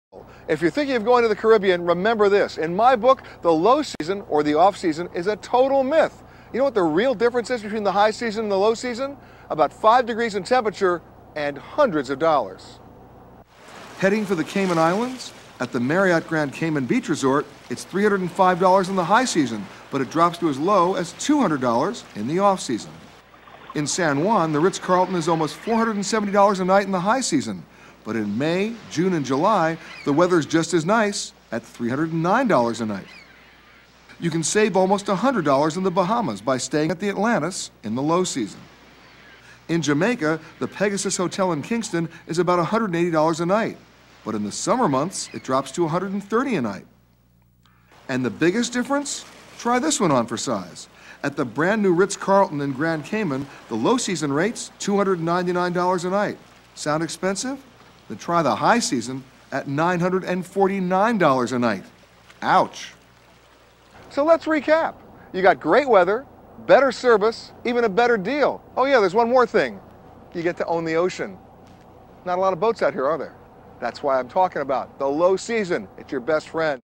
Caribbean High Season Vs. Low Season
More Travel Destinations...
For almost every travel destination, there's a high season and a low season. In some places, it's obvious why: no one wants to winter in Stockholm. In the Caribbean, the difference between the seasons is negligible--but the difference in prices definitely isn't! NBC Travel Editor Peter Greenberg shows you why.